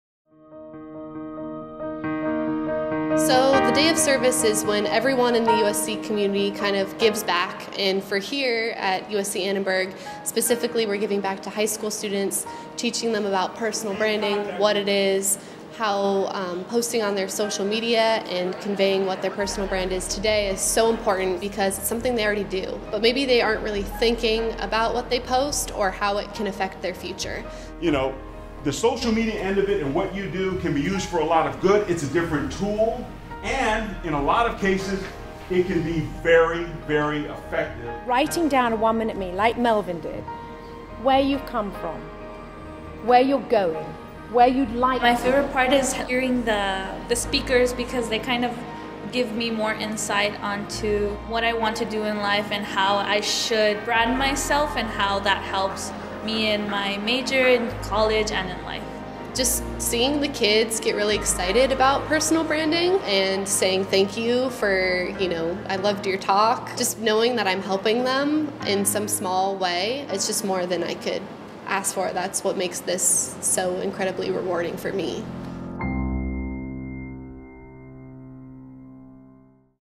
USC Alumni Day of SCervice with Annenberg
The USC Alumni Day of SCervice is an opportunity for all alumni and friends to participate in local service volunteer projects organized by our Trojan volunteers across the university.

USC Annenberg hosted a morning workshop on “Learning the Power of Branding." Alumni volunteers conducted mini professional development/mentoring sessions with local high school students on learning to develop their personal brand.